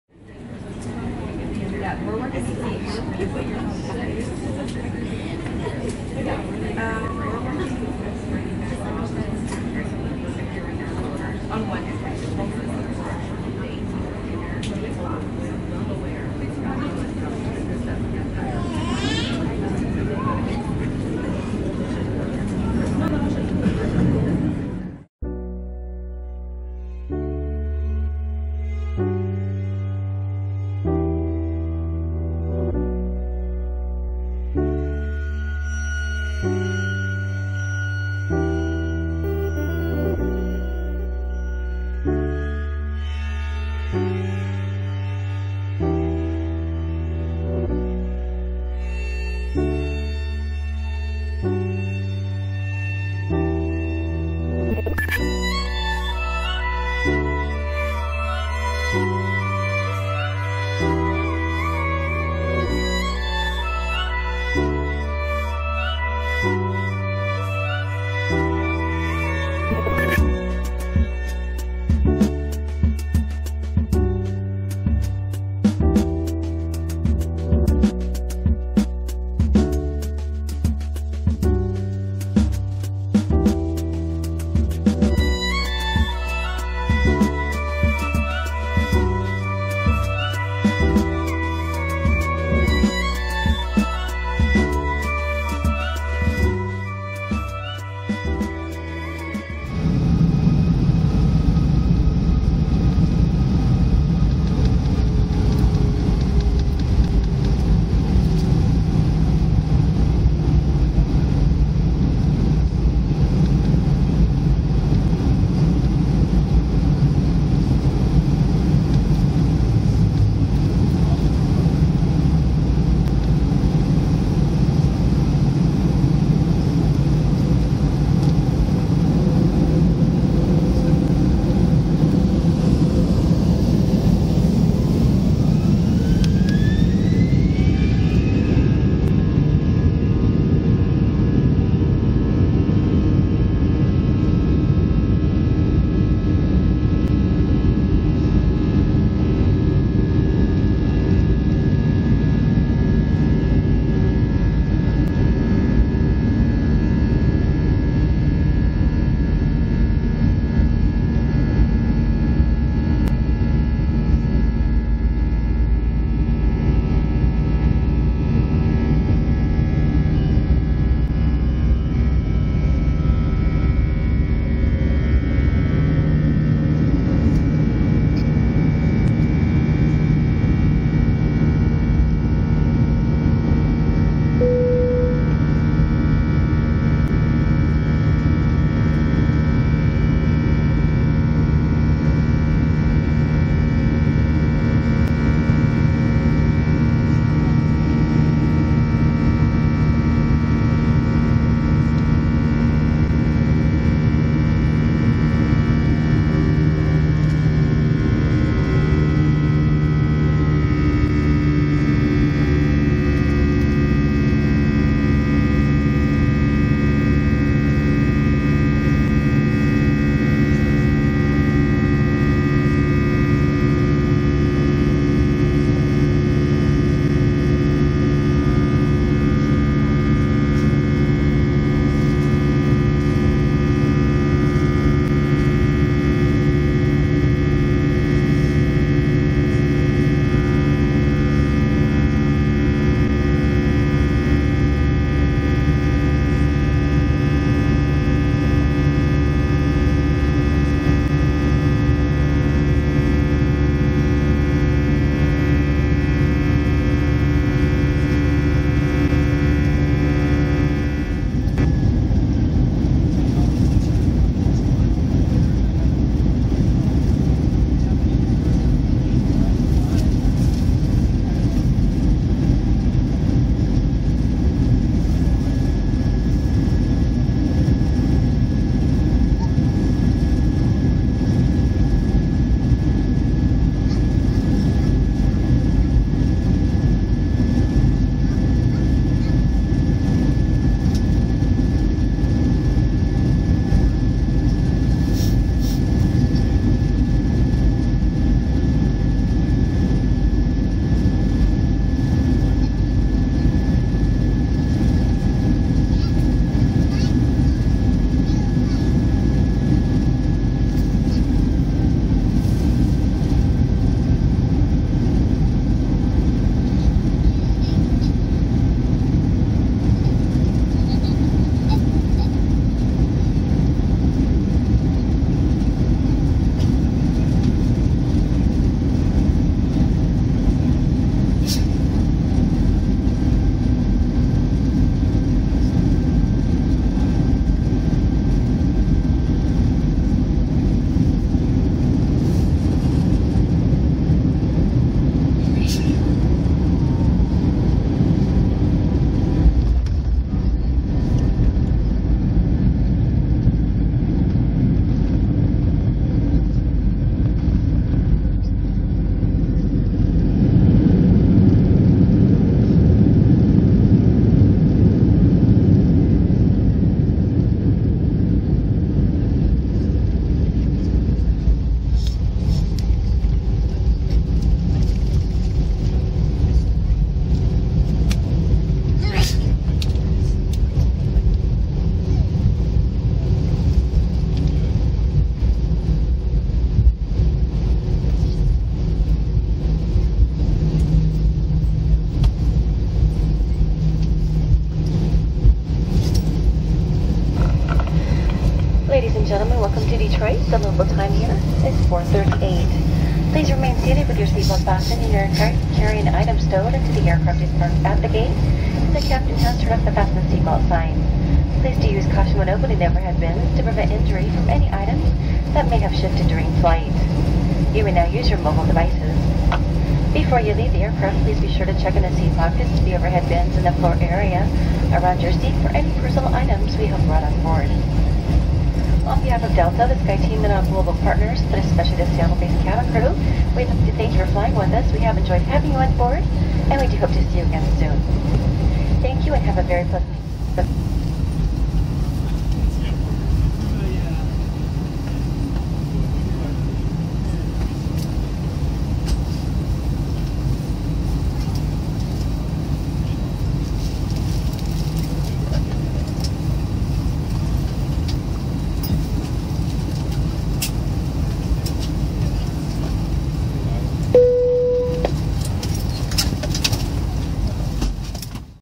Delta A330-200 Seattle Take-Off - Detroit Landing
Delta Air Lines flight 38 flew by Airbus A320-211
Take-Off from runway 34R, Detroit Seattle–Tacoma International Airport  (SEA)
Landing at runway 22R, Detroit Metropolitan Wayne County Airport (DTW)
Taken from seat 4D, filmed 11 January 2017

00:00 Boarding Gate
00:25 Taxi
1:40 Take-off
4:22 Landing
7:21 Taxing to Gate